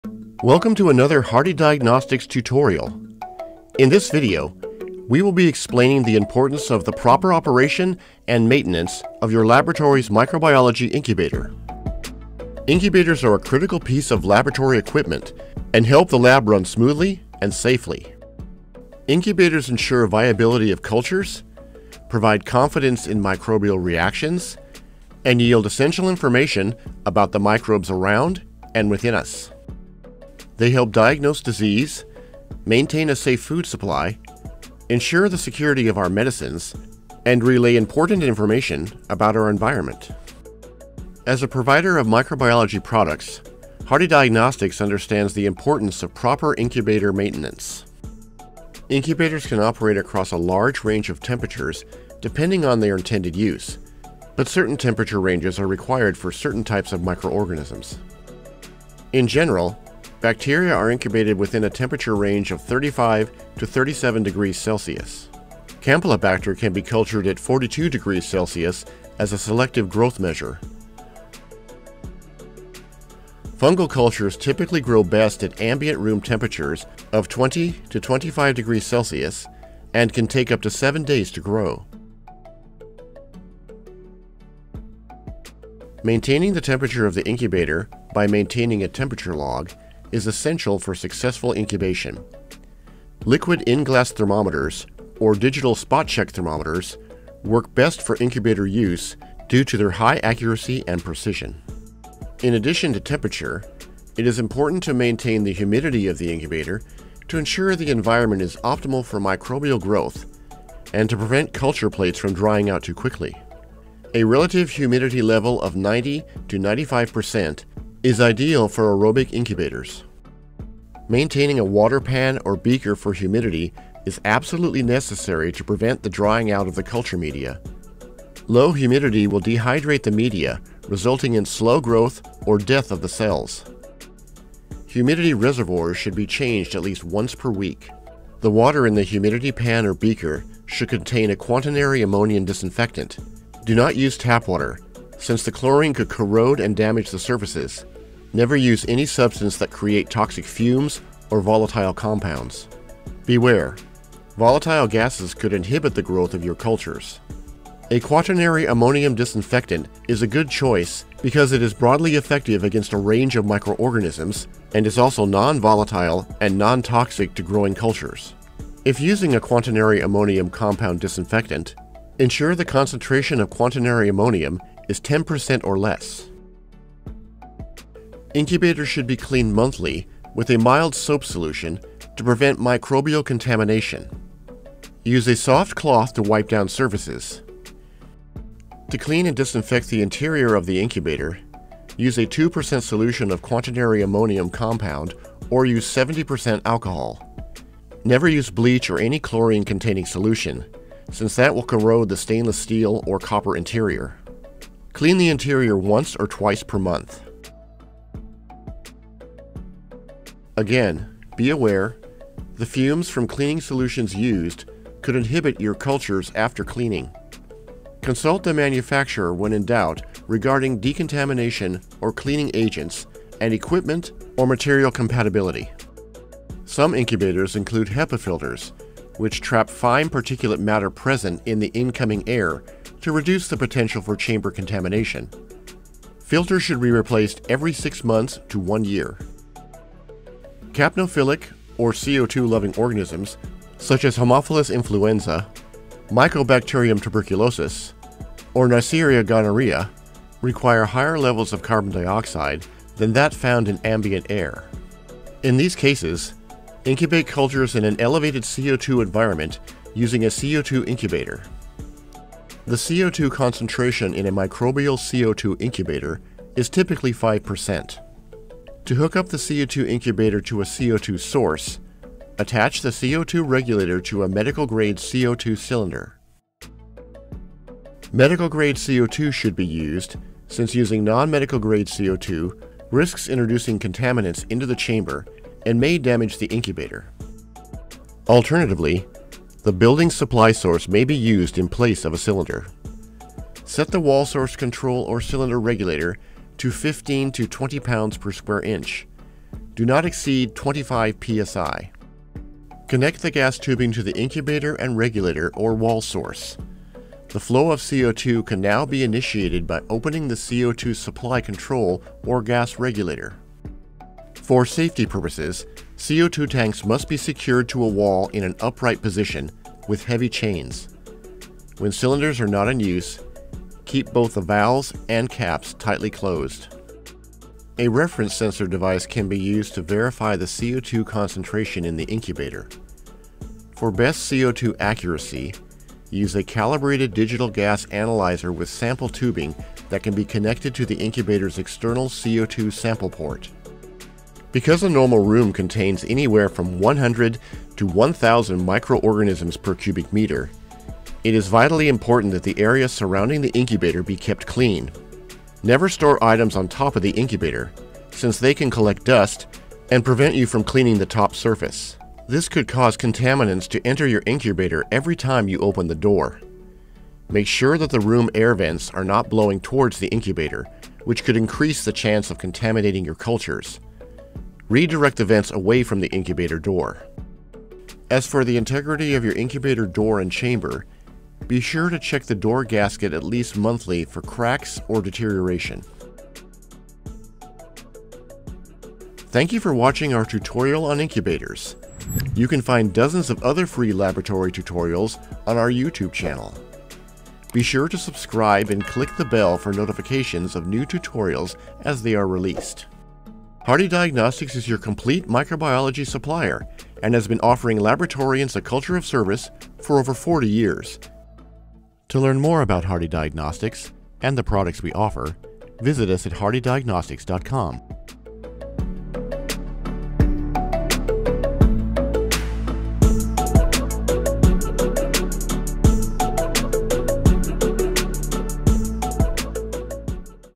Care & Cleaning - How to Maintain the Laboratory’s Microbiology Incubator - Hardy Diagnostics
An incubator is essential in the microbiology laboratory. The incubator is a heated, insulated box used to grow and nurture both bacterial and eukaryotic cells. As these elements of human or animal bodies require controlled and regulated temperature and humidity conditions, specialized laboratory equipment is used. The incubator maintains optimal temperature, humidity and other conditions such as the CO2 and oxygen content of the atmosphere inside.  As a provider of microbiology products, Hardy Diagnostics understands the importance of proper incubator maintenance.

Featured Products:

IncuPRO Incubator, LCD Temperature Indicator, Digital, 0.8 cubic feet Cat. no. 133000H

Incubator, 0.8 Cubic Feet, Medium, with Acrylic Plastic Door, with Thermometer Cat. no.132000

Heratherm™ IMC 18 Incubator Cat. no. 200513

HardyCHROM™ Campylobacter, Chromogenic Medium Cat. no. G339

HardyFlask™ Potato Flake Agar, 12ml Cat. no. X32

Martin Lewis with Lincomycin, for Neisseria gonorrhoeae Cat. no. E39

00:00 Intro
00:13 Incubators' importance
00:48 Temperature ranges
01:42 Humidity
02:57 Cleaning
04:00 CO2 loving organisms
04:22 CO2 Incubators
05:43 Additional incubator maintenance
06:28 Thank You

Filmed, Directed, and Edited by Brian Tom